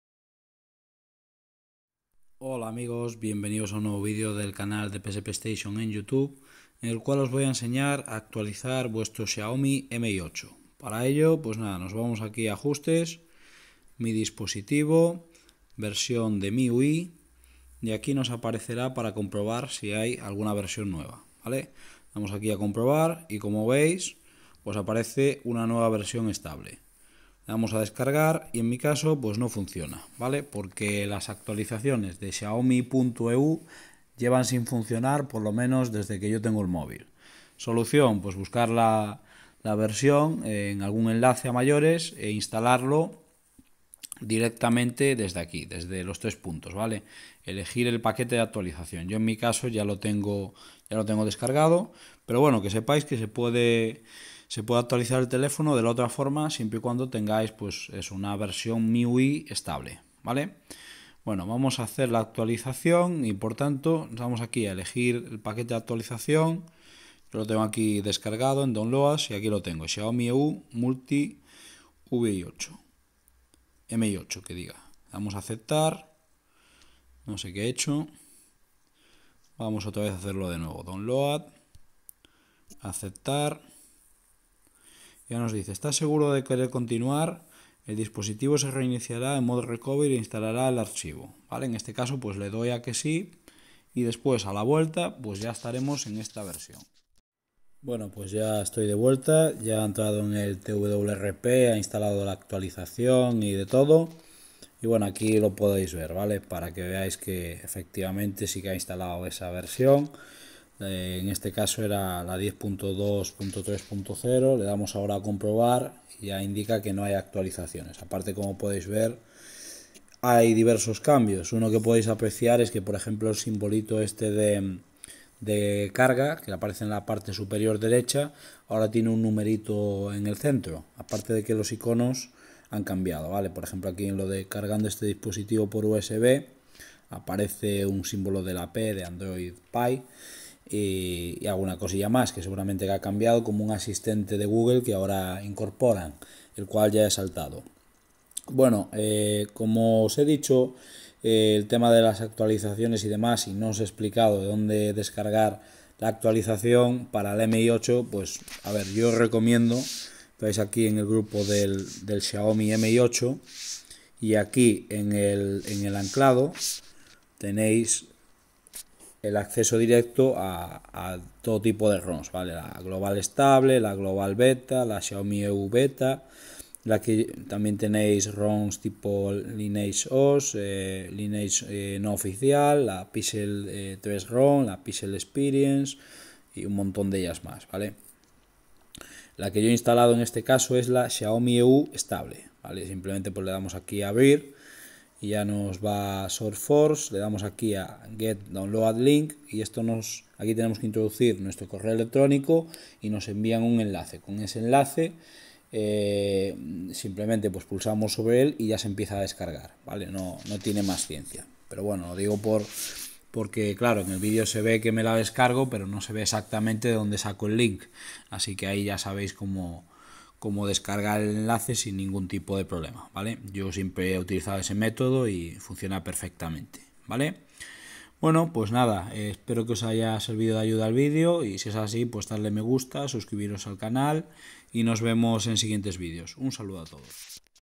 Cómo actualizar el Xiaomi Mi 8
En este vídeo te enseño a actualizar a actualizar el Xiaomi Mi 8. Pulsa sobre este enlace para acceder al grupo de Telegram
- Comprar Xiaomi Mi 8

El anterior es un enlace de afiliación. Comprando a través de este enlace ayudas al canal y recibes ningún carga a mayores. Muchas gracias!